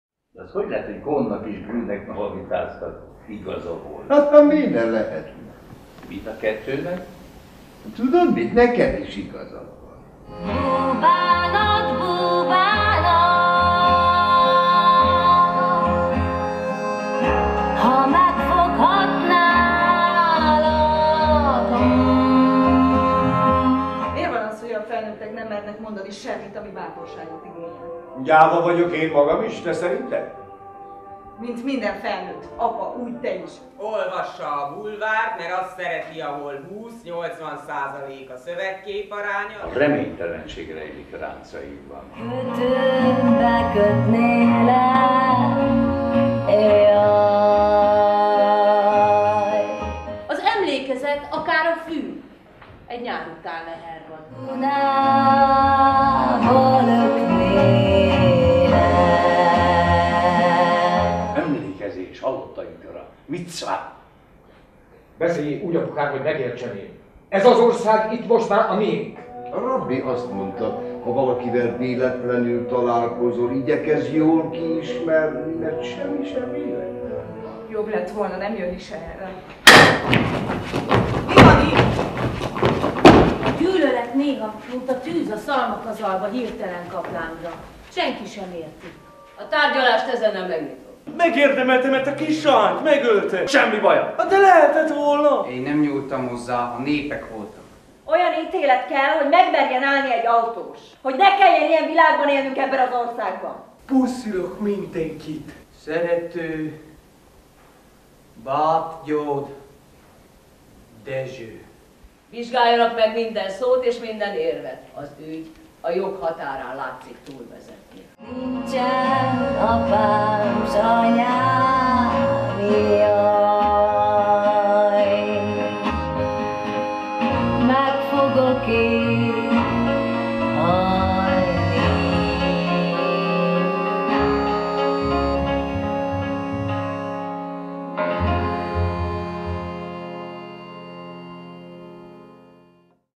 Az Olaszliszkai - ajánló
Borbély Szilárd Az Olaszliszkai c. drámáját 2015. október 9-én mutatja be a Katona József Színház, Máté Gábor rendezésében. Videós ajánló. Készítette: Kazimir Annamari